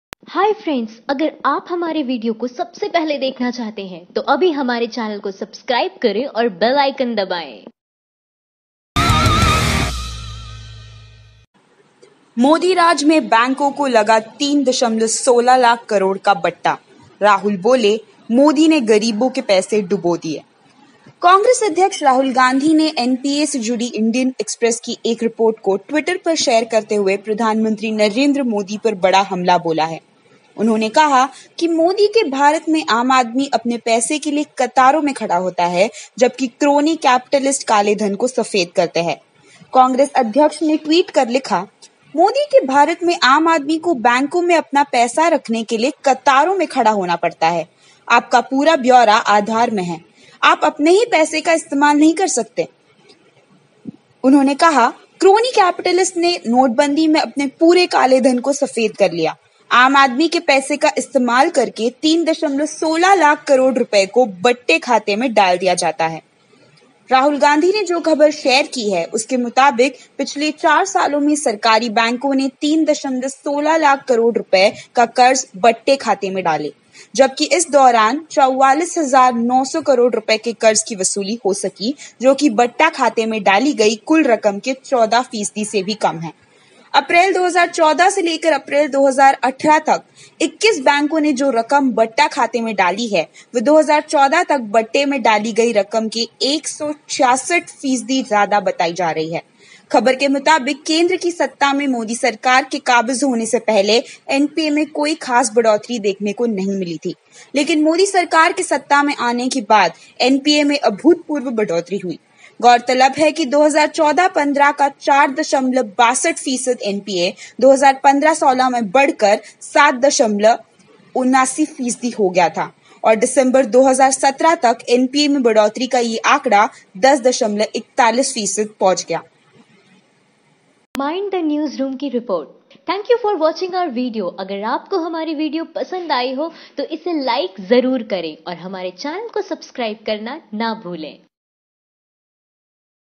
Modiराज में बैंको को लगा 3.16 लाख करोड़ का बट्टा, BJP और Modi सरकार की पोल खोल- Part 96 of 200.
-~-~~-~~~-~~-~- BJPLatestNewsinHindi.